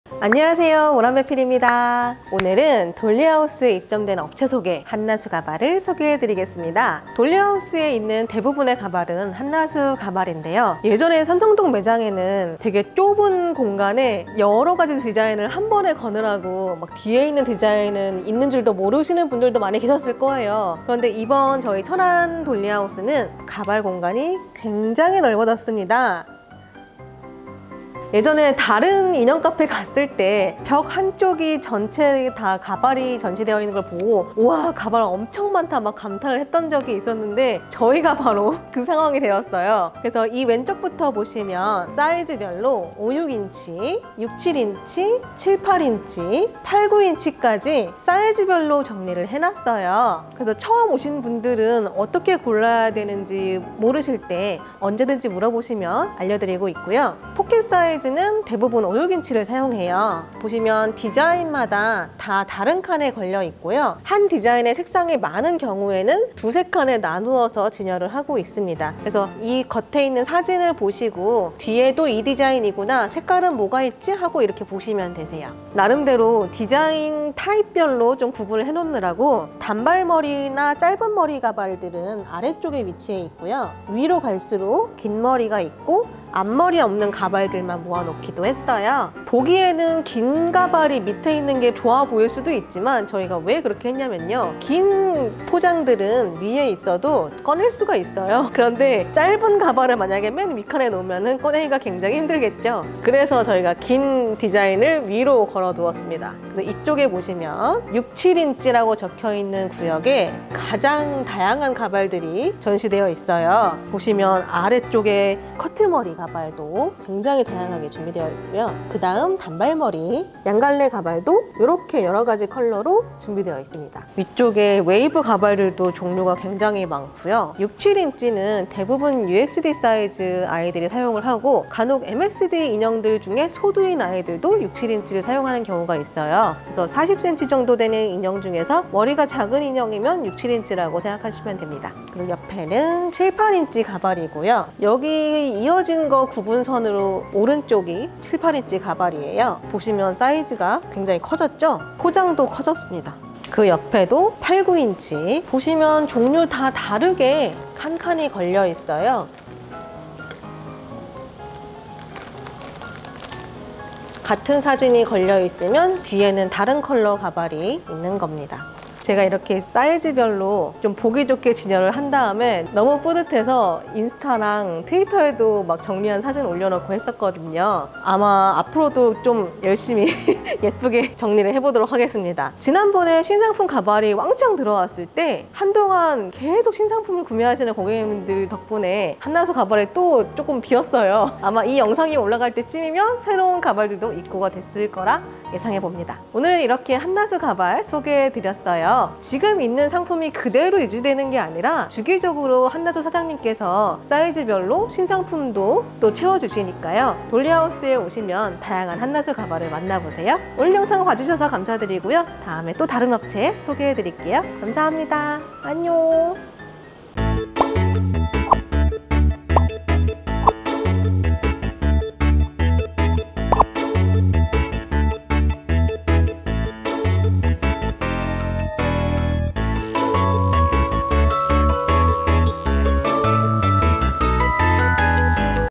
가발맛집 한나수 가발을 소개합니다~!
돌리하우스 가발존 벽면을 꽉 채운 한나수 가발!
어떤 사이즈 가발들이 있는지, 어떻게 진열되어 있는지 자세히 알려드릴께요~